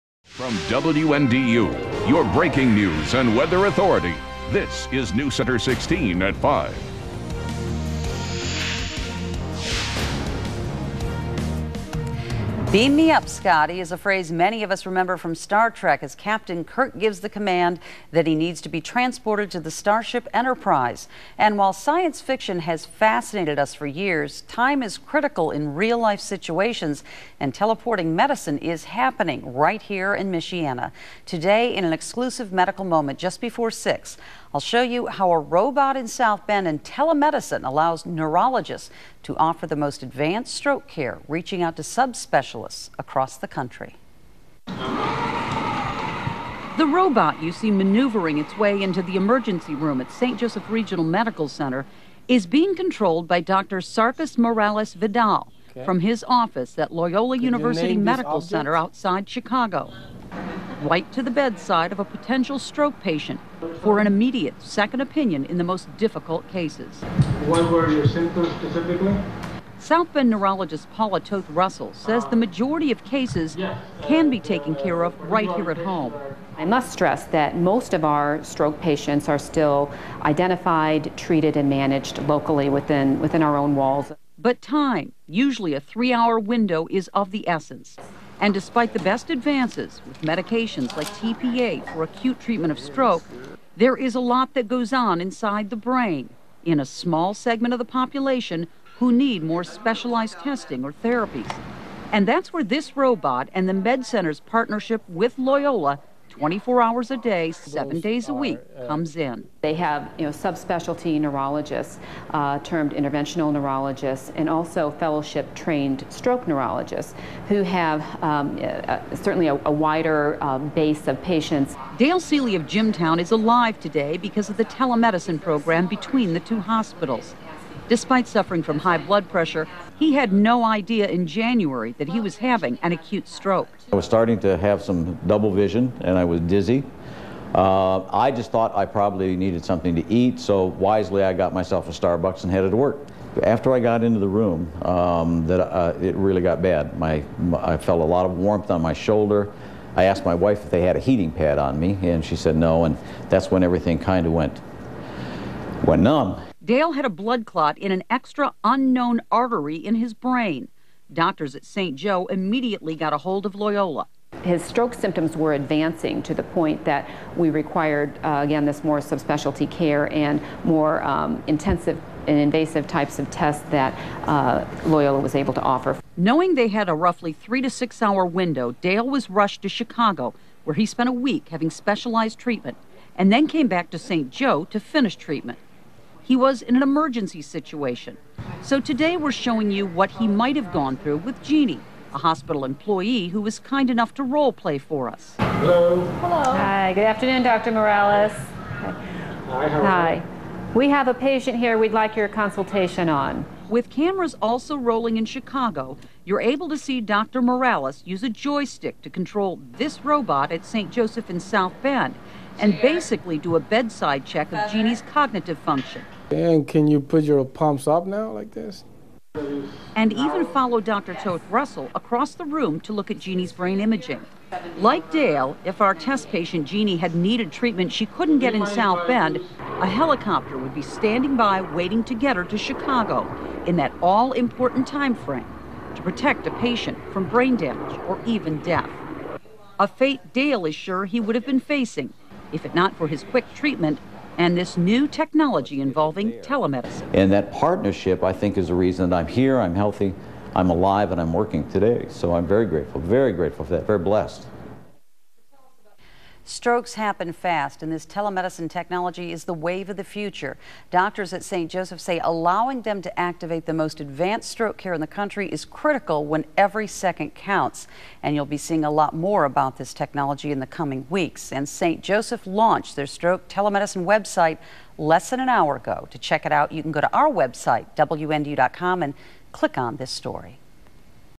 Maureens's Medical Moment: Stroke Telemedicine
A stroke happens fast, and when one occurs, every second counts. That's why Saint Joseph Regional Medical Center continues to invest in state-of-the-art technology to ensure that the community has access to the most advanced stroke care in the country. As the first hospital in the region with a Primary Stroke Unit certified by the Joint Commission, our Code Stroke emergency activation system is in place to ensure that patients who exhibit signs of a stroke, receive the fastest possible treatment. Now, through a new partnership between Saint Joseph Regional Medical Center and Loyola University Medical Center, SJRMC will continue to lead the charge in stroke care by developing the first stroke telemedicine program in Northern Indiana.